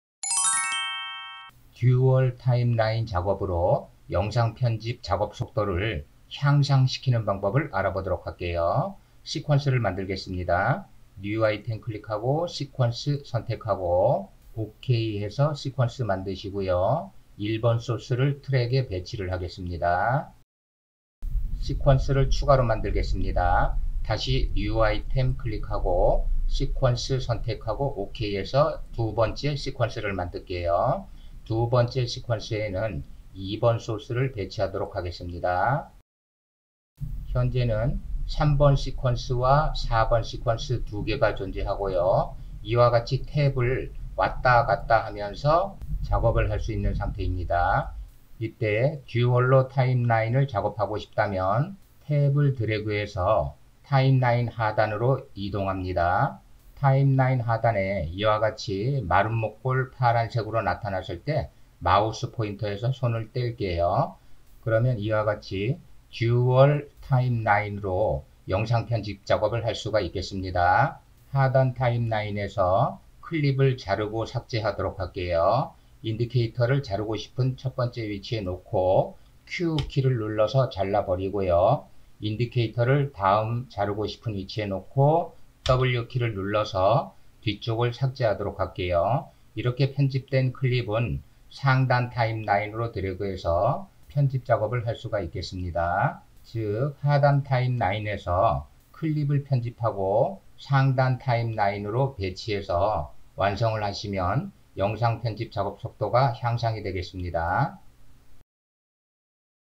[프리미어] 듀얼 타임라인 패널 운용으로 영상 편집 작업 속도를 향상 시키는 방법
1:02 오디오 정정: 마우스 포인터 → 마우스 버튼